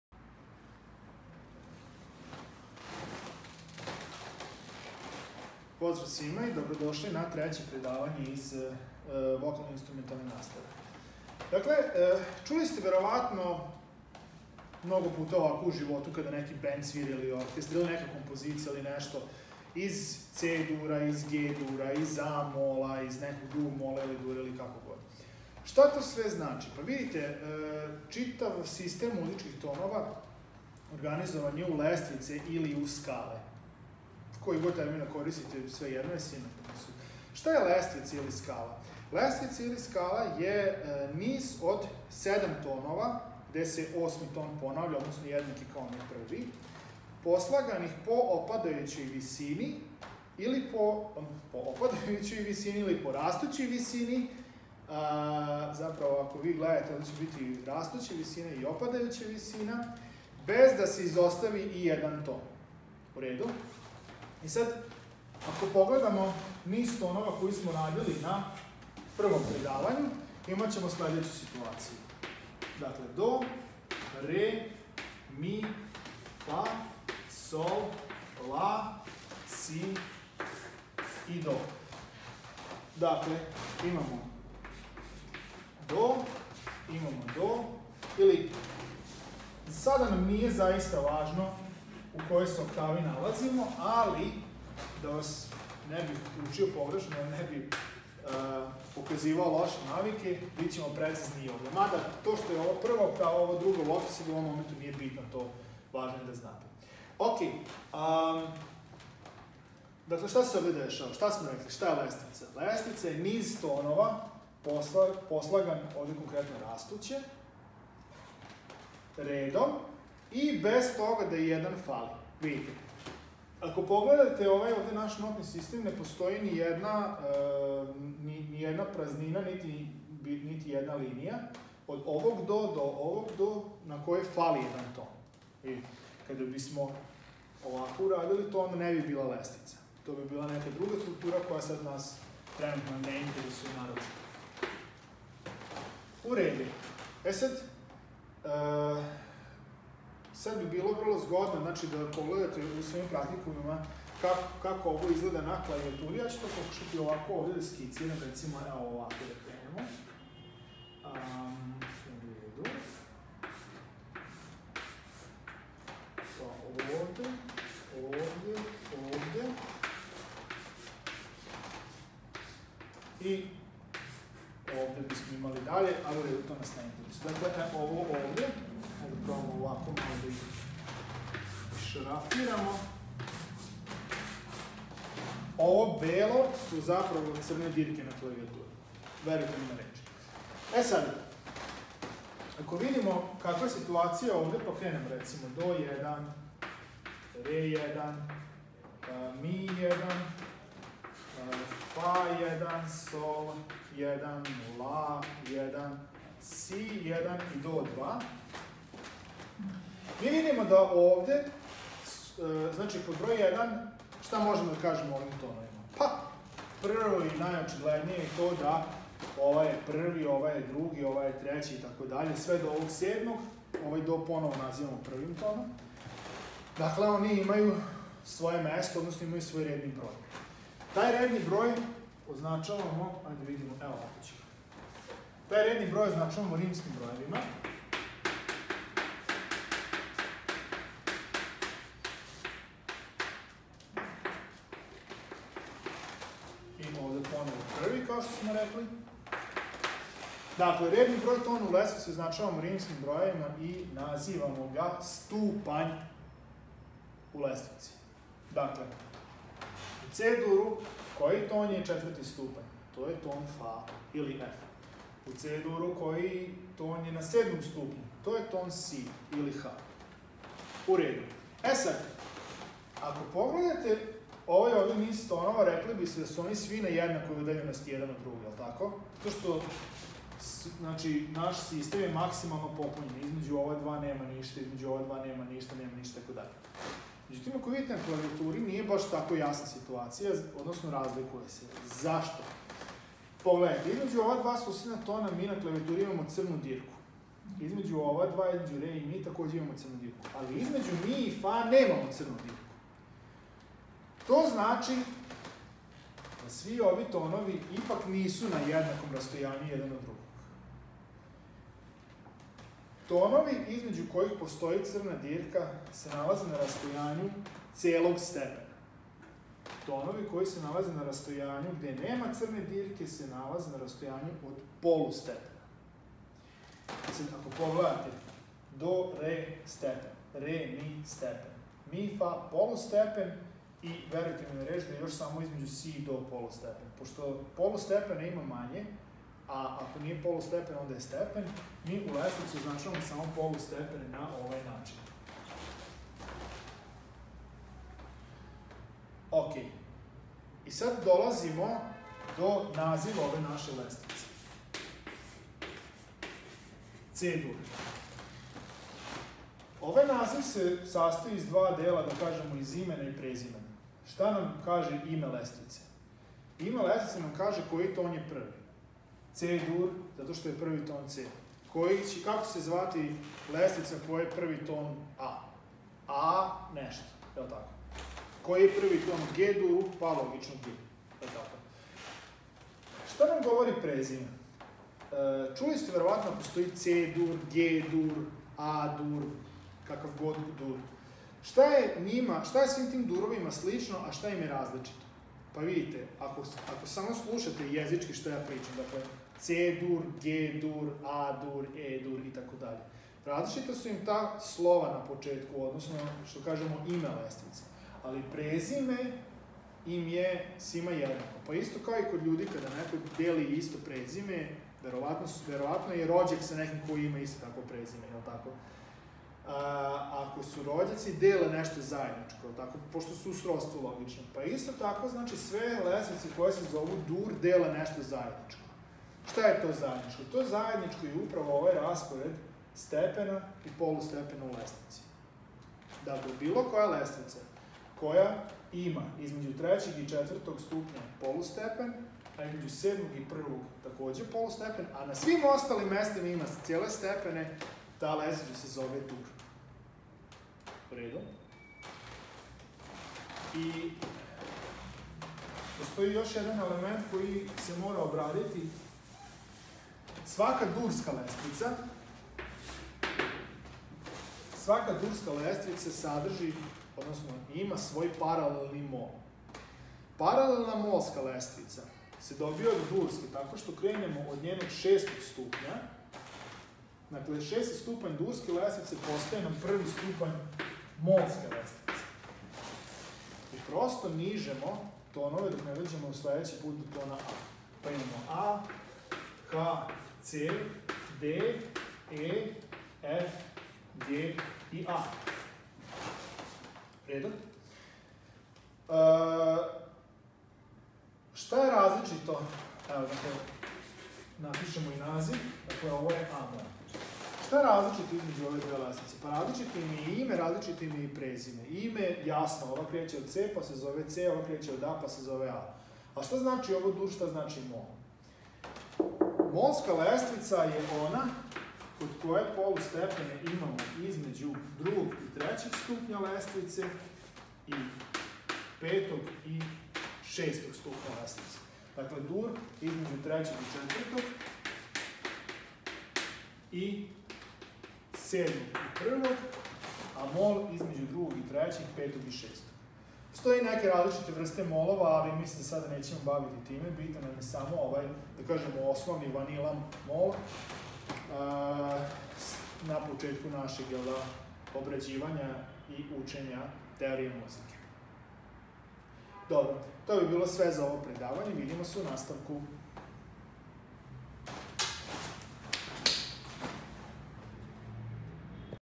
[Vokalno instrumentalna nastava] Predavanje #3
Visoka škola strukovnih studija za obrazovanje vaspitača u Novom Sadu
Predmet: Vokalno instrumentalna nastava
Predavanje 3 : Lestvica - jedinica organizacije tonova. Lestvični stupnjevi. C Dur i a moll lestvica. Stepeni i polustepeni.
Izvodi dr Nikola Vetnić.